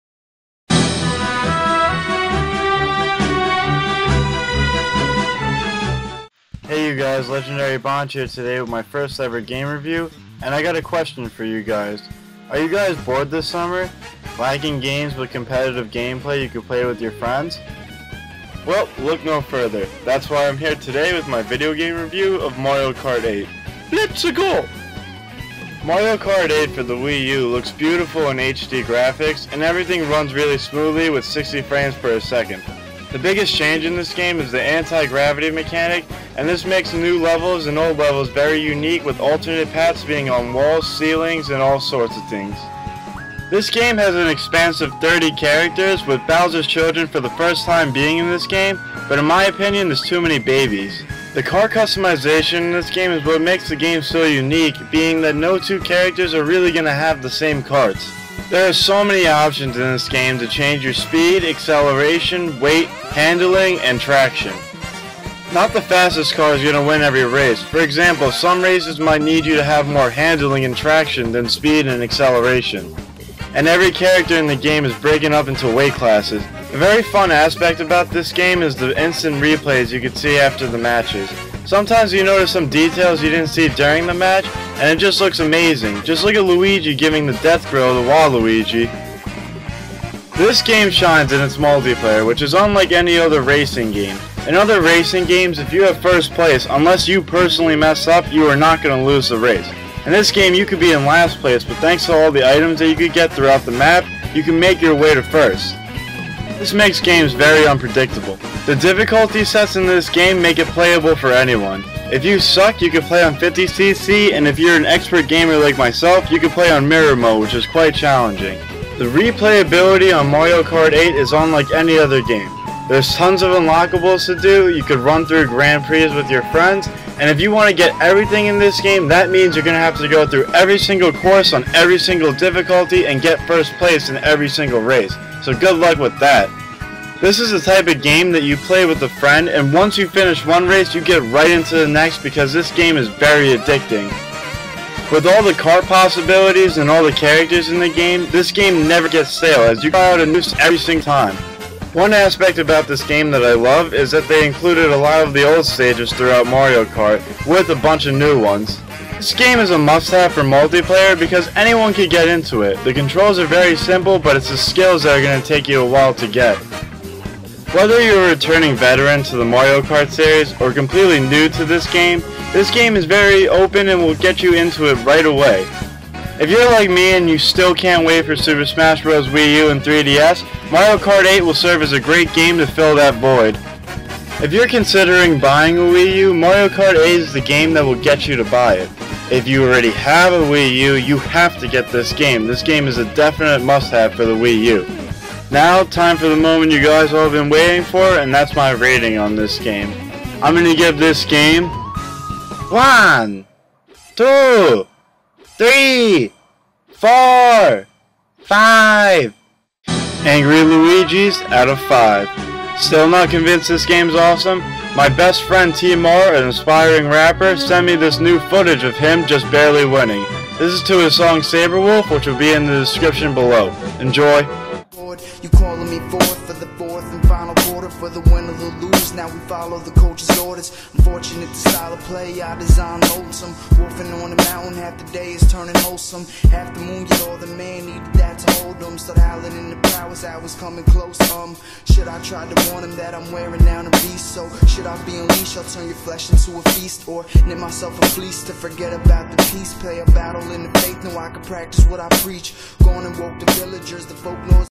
Mario Kart 8: Review
Mario Kart 8 recently released for the Wii U is a must have game! I strongly recommend you guys to get or at least play this game!